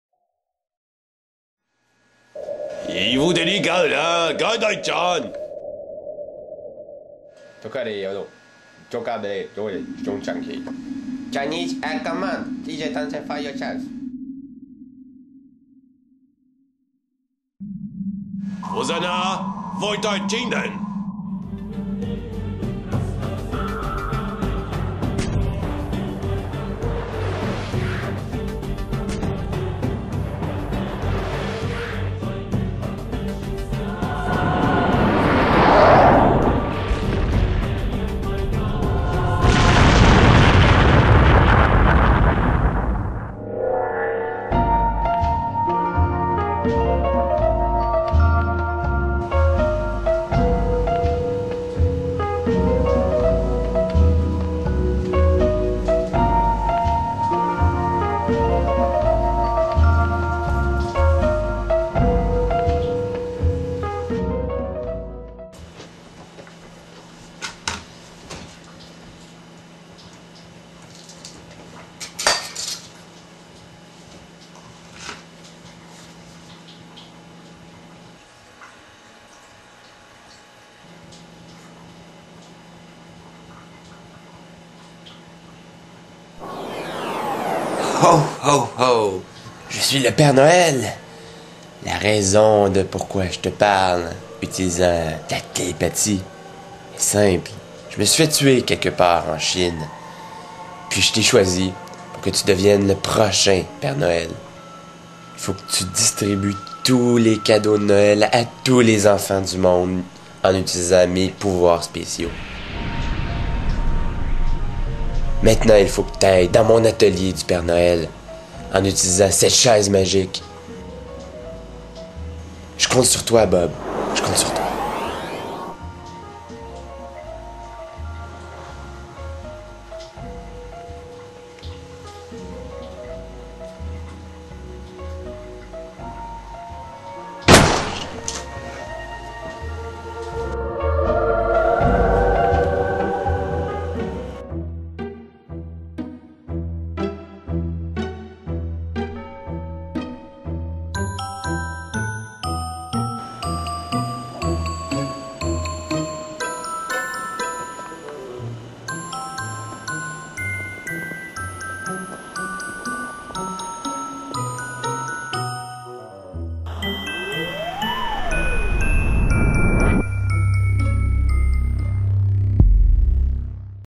Le Père Noel est mort
Que se passerait-il si le Père Noel se faisait tirer un missile par les chinois. Réponse dans cet heureux et positif vidéo. Vous avez probablement compris que le vidéo n'est pas positif du tout. Soumis en premier à filmfights.com et ensuite rendu disponible dans la section anglaise du site, je vous l'offre maintenant en version originale française. Le vidéo fut très très apprécié pour son humour noir et ses effets spéciaux.